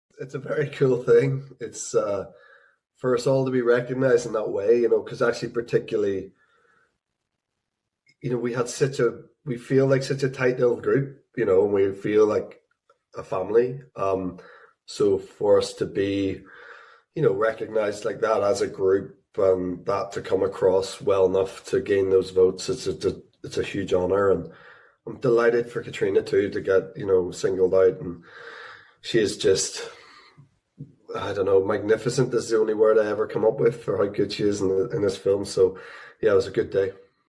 Jamie Dornan 14/01/22_(ETCanada)
Jamie Dornan at home (ETCanada interview about  caitríona balfe)